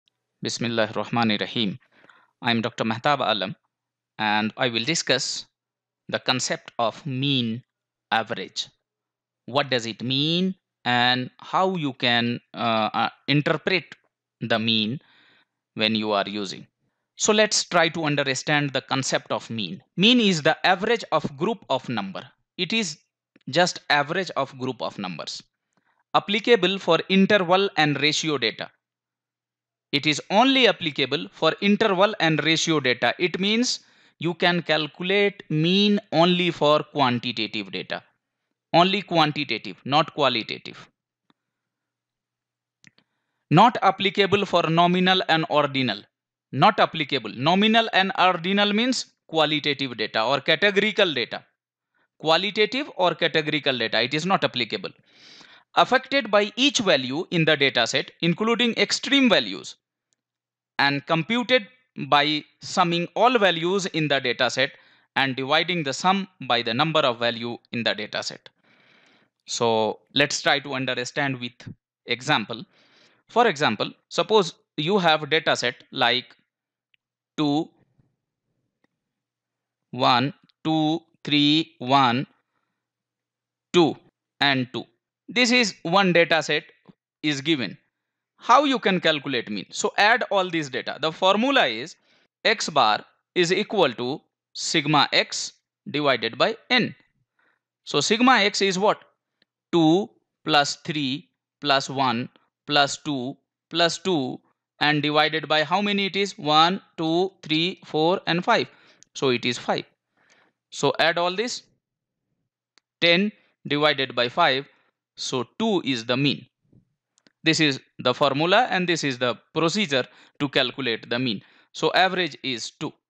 Applied Biostatistics | U3-L4 | Mean / Average
Chapter 3 - Lecture 4 : Mean / Average
Meaning and Concepts of mean and average
Types of data and possible calculation of mean
Numerical Example to calculate mean
Steps and Formula to calculate mean for raw data